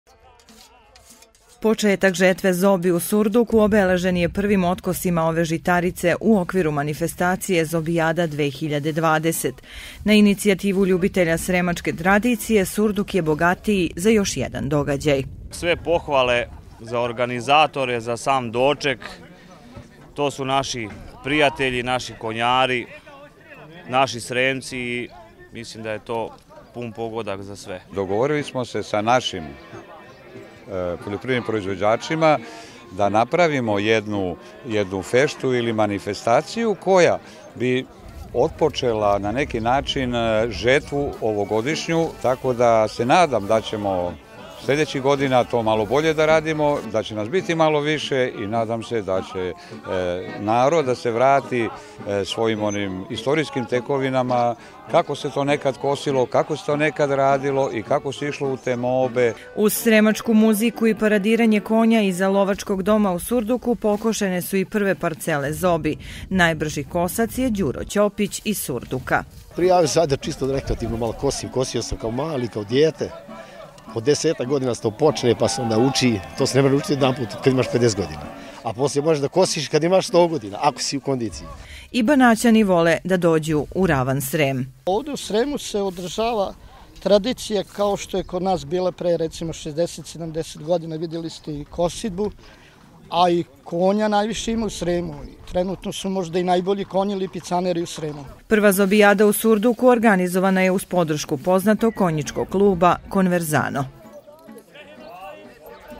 Prva Zobijada u Surduku
Početak žetve zobi u Surduku danas je obeležen prvim otkosima ove žitarice u okviru manifestacije  Zobijada 2020. Uz sremačku muziku i paradiranje konja  iza  Lovačkog doma pokošene su i prve parcele zobi. Najbrži  kosac je Surdučanin Đuro Ćopić. Prva Zobijada u Surduku organizovana je uz podršku poznatog Konjičkog Kluba "Konverzano".